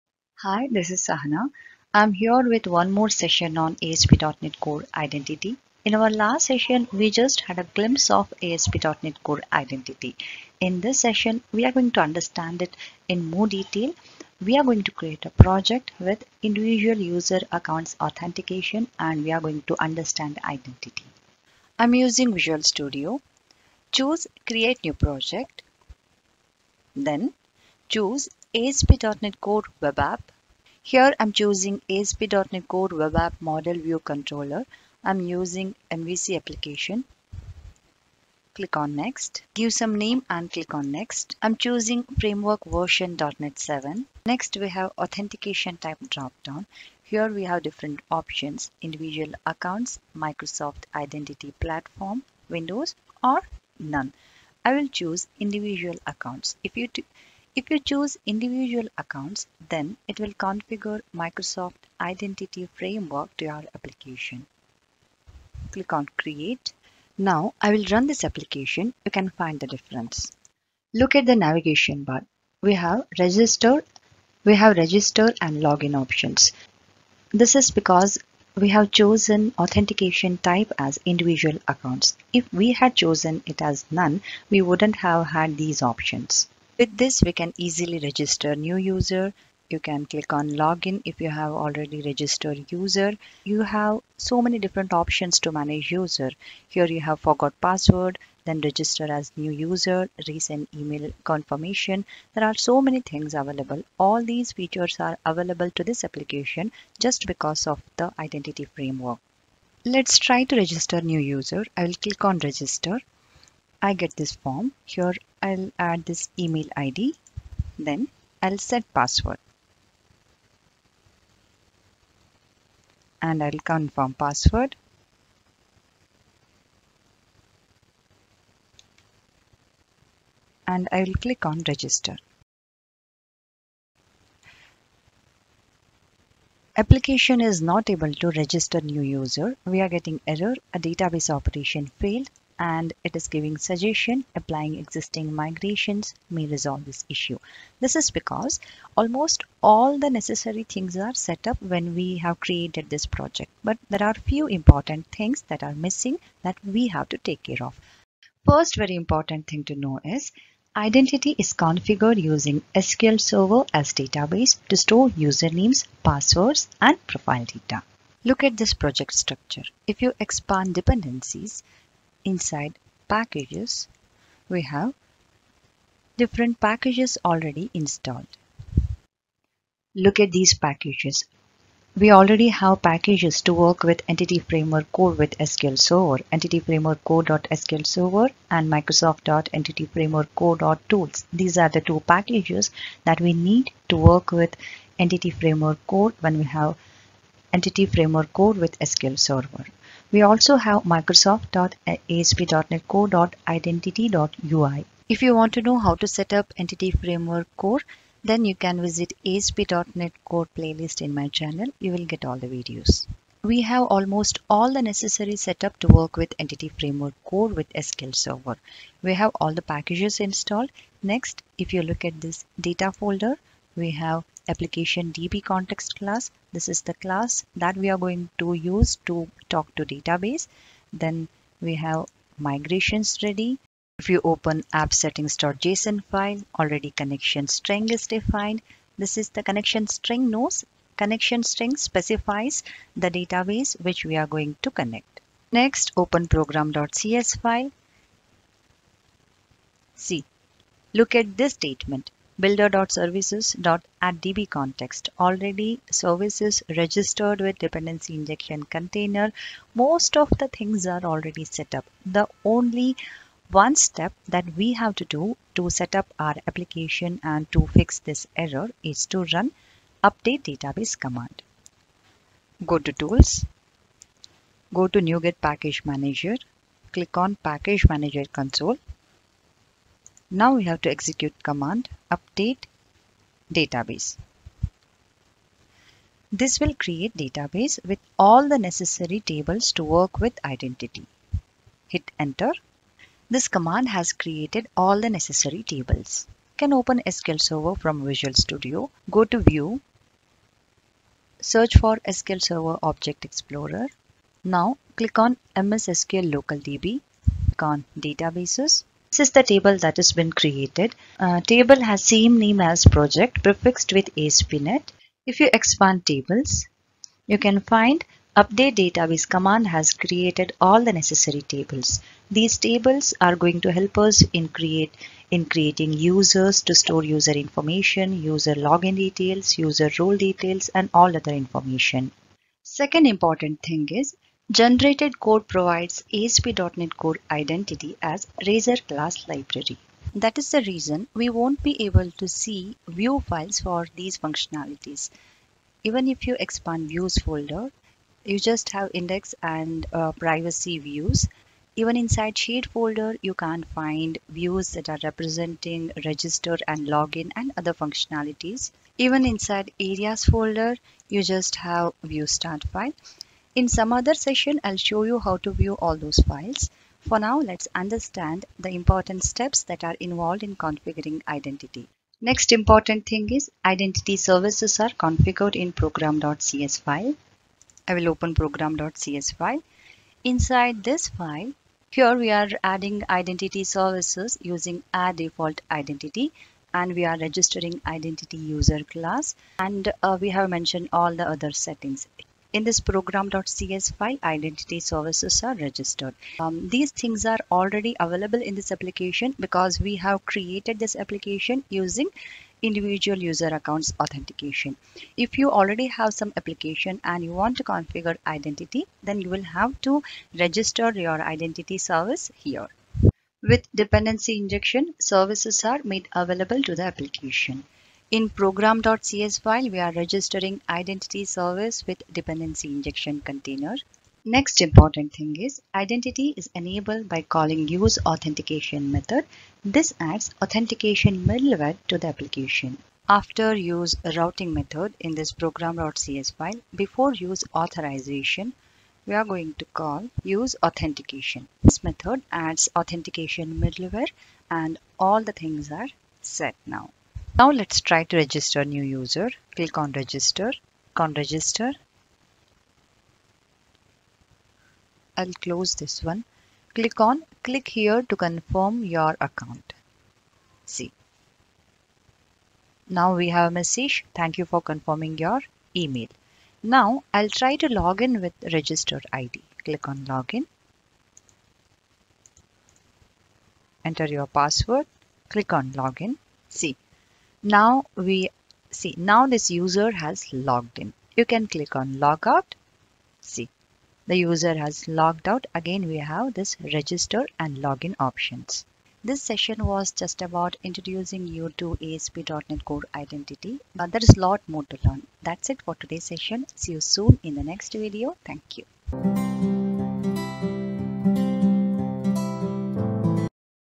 Create a project with asp.net core Identity | Asp.net core Security
In this video you will understand what is asp.net core Identity? And Why do we use asp.net core Identity? How to create a project or application with Asp.net core Identity using template available - Individual user authentication